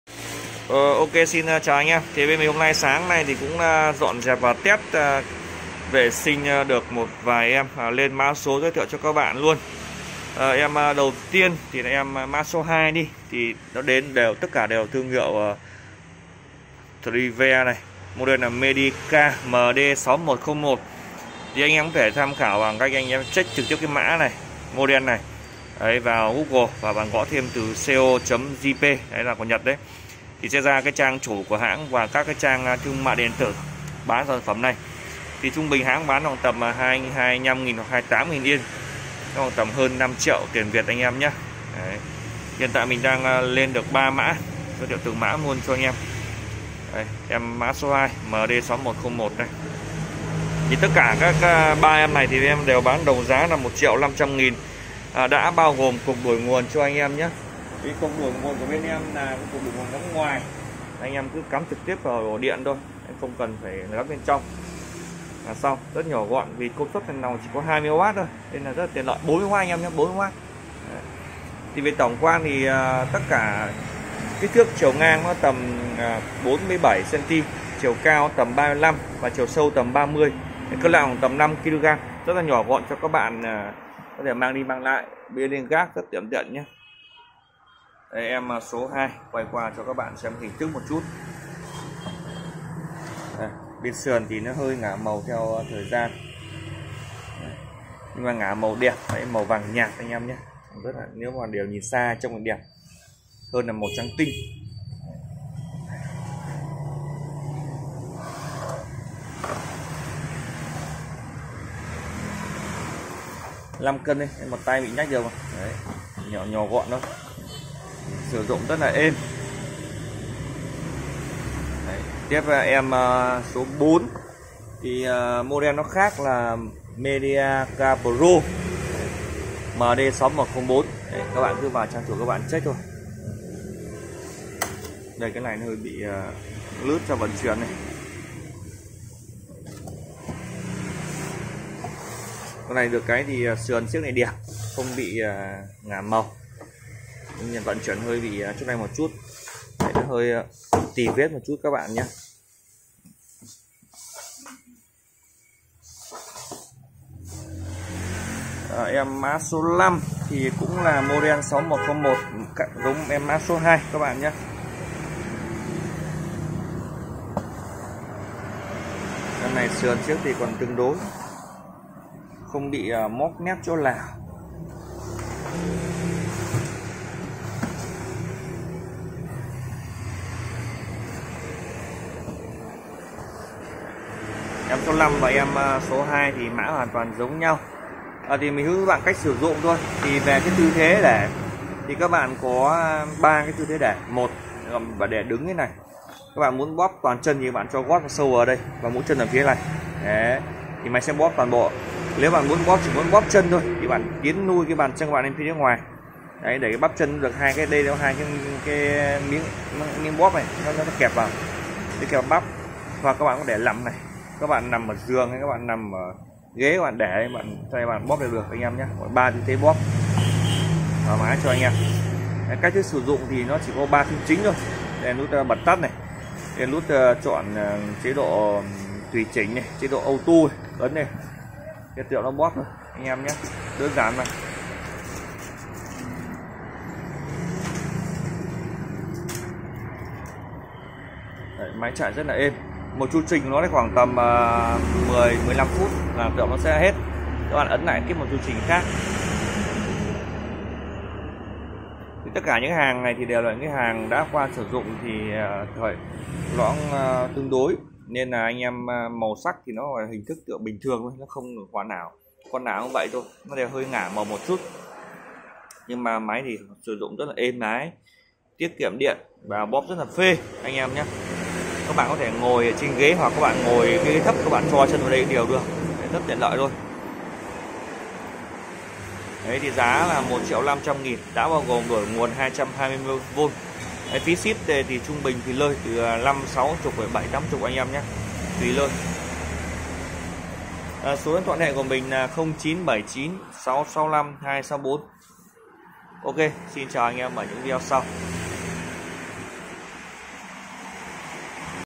Máy mátxa chân Thrive nội địa Nhật.Giá 1.5t cả đổi điện 220v
Đỉa chỉ:
Cs 2: Số 3 ngõ 229 miếu 2 xã, Dư Hàng Kênh, Lê Chân, Hải Phòng (cạnh chợ hàng )
Cs 1: Số 141 quán rẽ, Mỹ Đức, An Lão, Hải Phòng ( Cạnh Cầu Nguyệt Kiến An )

Nhóm zalo đồ bãi, a e kết nối nhé.